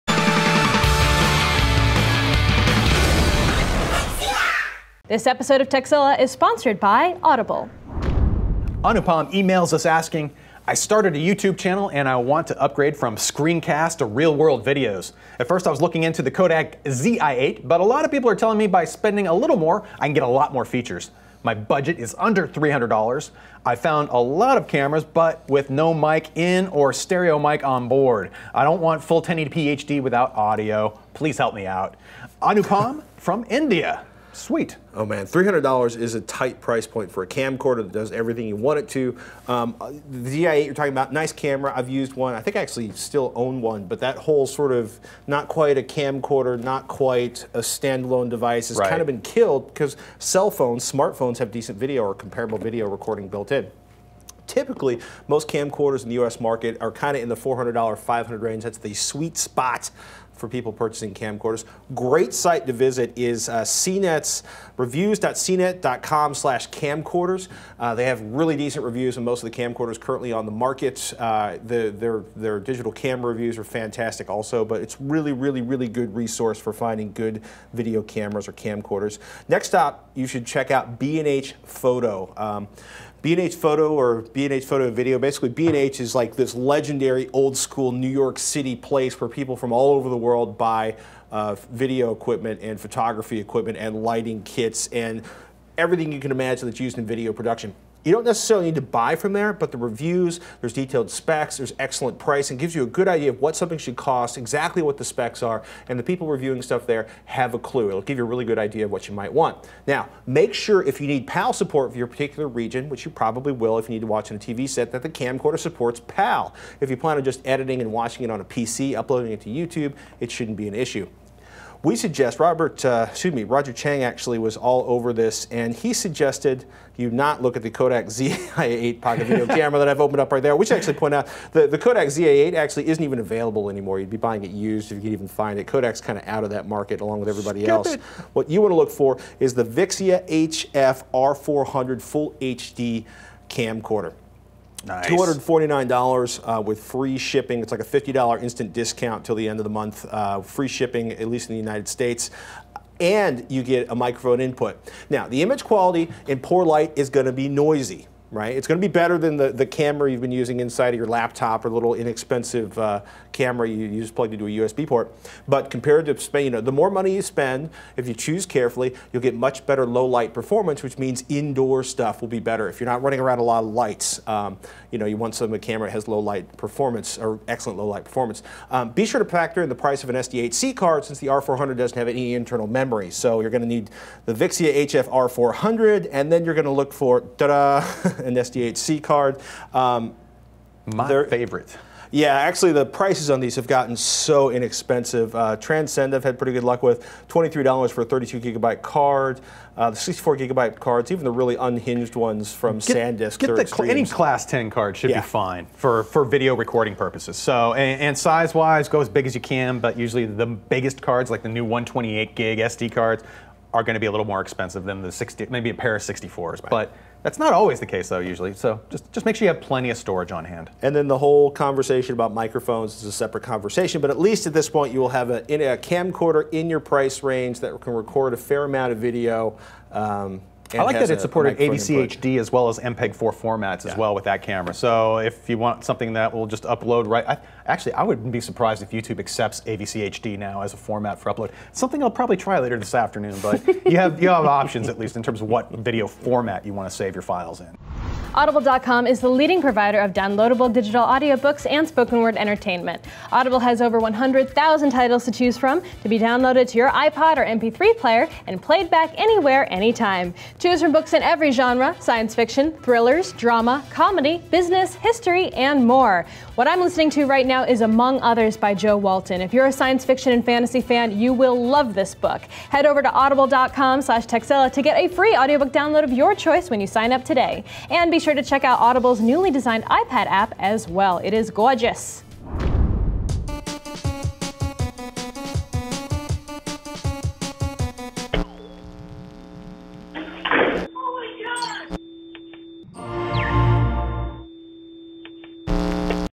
Best Sub-$300 Camcorder Pick
Can we find a sub $300 video camera with audio in jacks? We pick the Canon VIXIA HF R400. A great place to check out camera reviews is over at CNET and for basic camera features, take a look at BHPHoto.